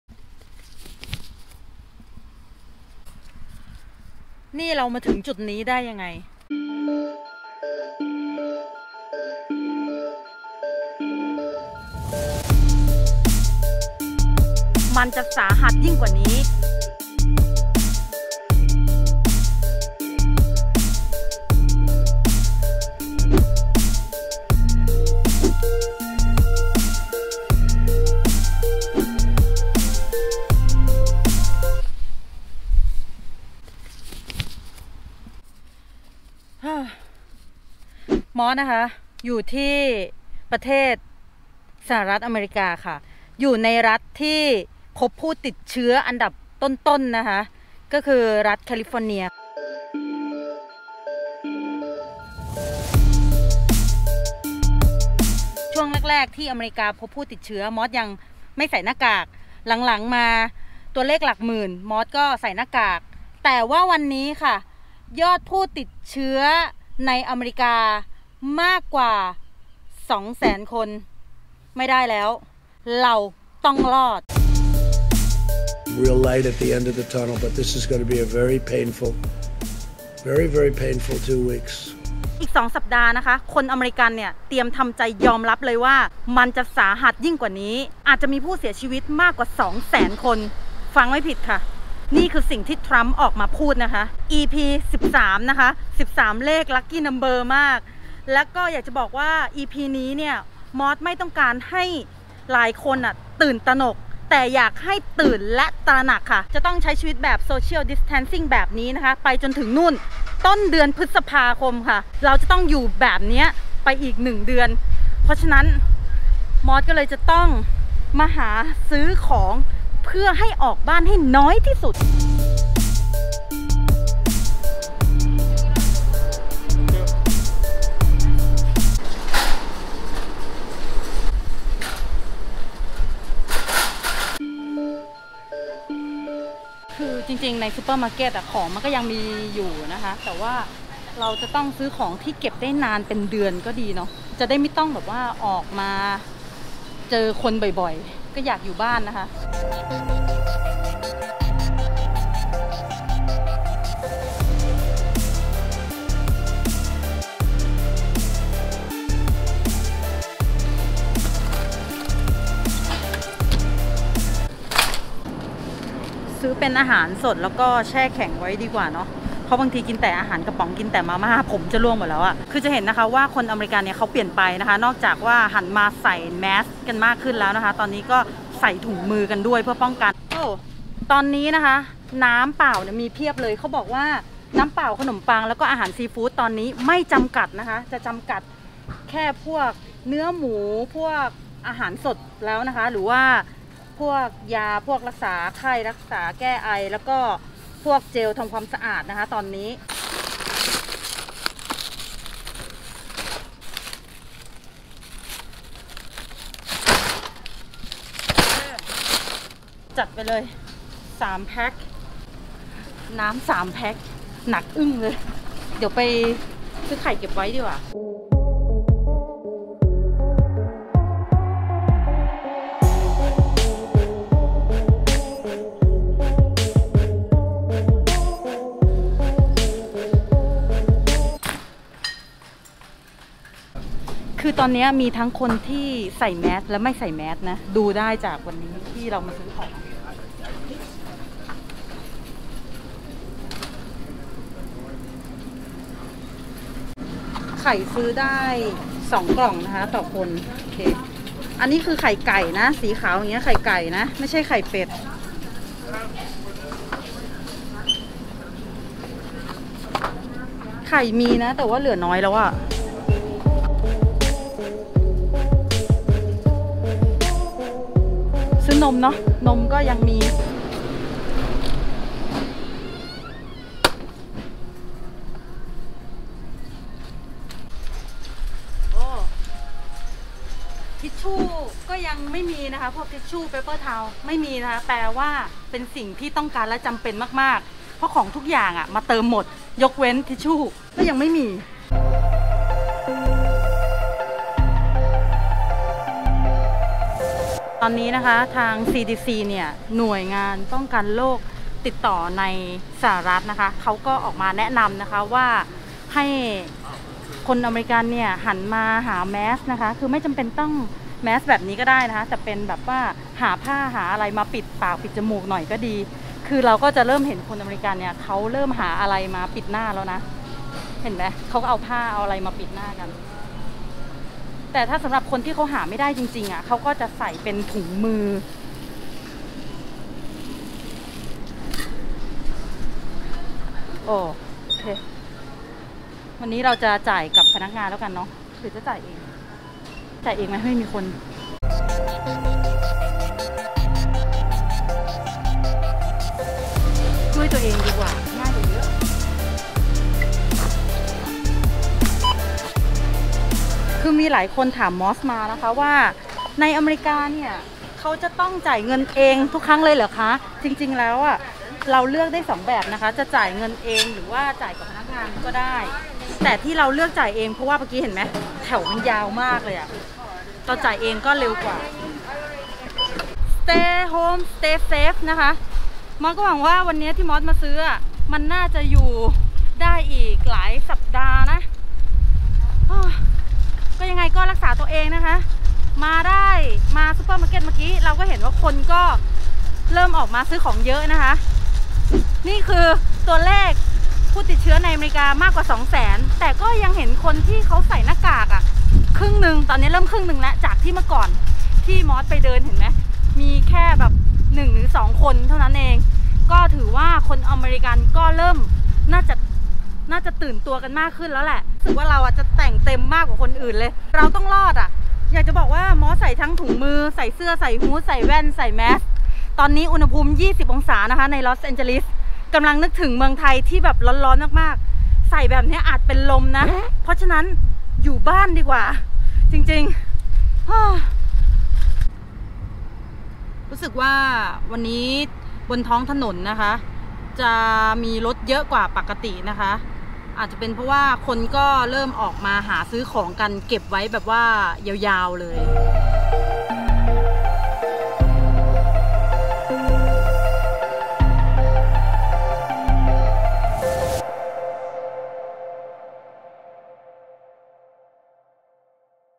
อเมริกาสาหัส! ทะลุ2แสนคน |สถานการณ์ในอเมริกาตอนนี้ EP13 มอสลา 01/04/20
อเมริกาเปลี่ยนไป!? คนอเมริกันเป็นยังไง?

วันที่ 1 เมษายน 2020 (เวลาท้องถิ่นในLA)